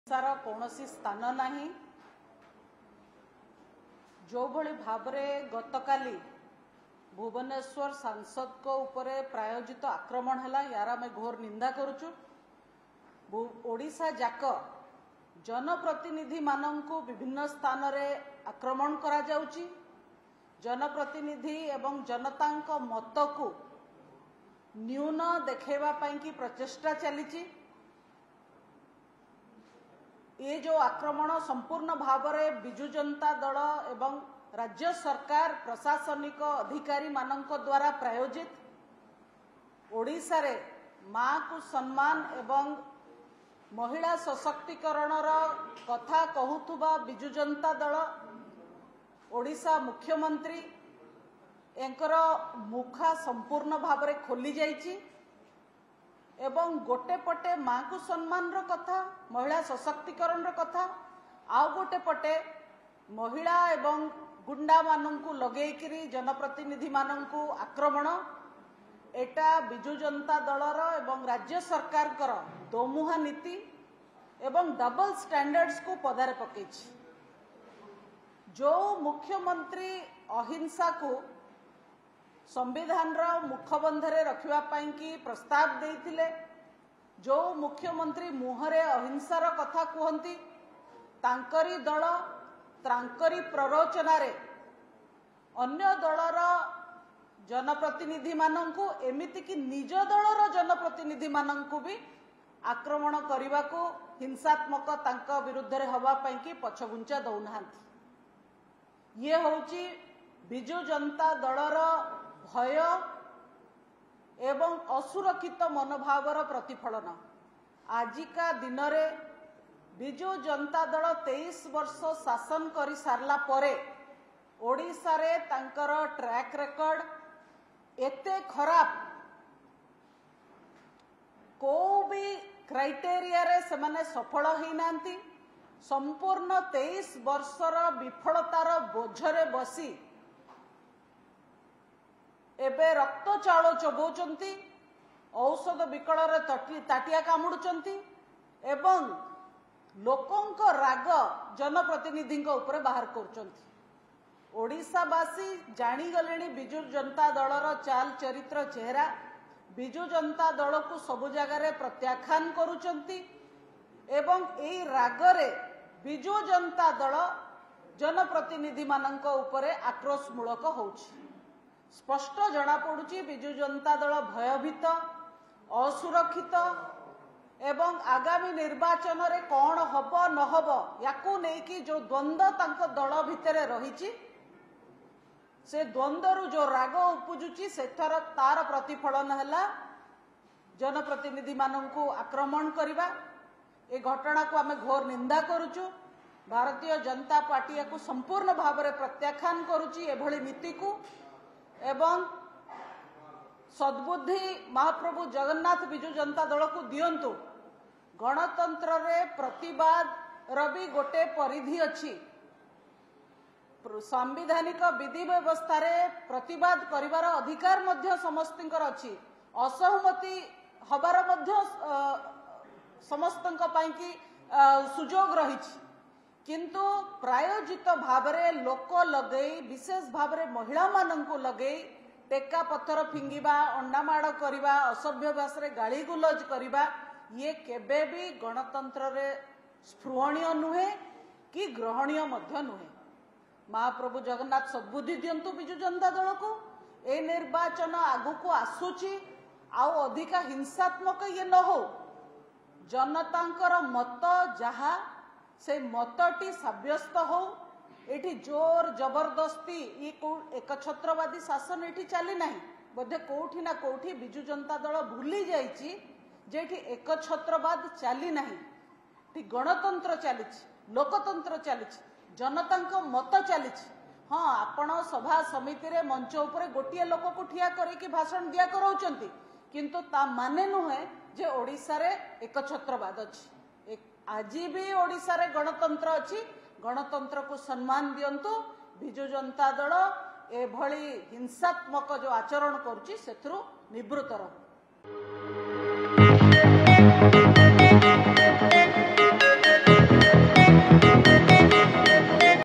BJD Attacked on MP Aparajita Sarangi ‘ସାଂସଦଙ୍କୁ ବିଜେଡି ଆକ୍ରମଣ କରିଛି’ | Odisha Reporter
‘ସାଂସଦଙ୍କୁ ବିଜେଡି ଆକ୍ରମଣ କରିଛି’ କିଛି ଏଭଳି ଅଭିଯୋଗ କରିଛନ୍ତି ବିଜେପି ନେତ୍ରୀ ଲେଖାଶ୍ରୀ ସାମନ୍ତ ସିଂହାର  ।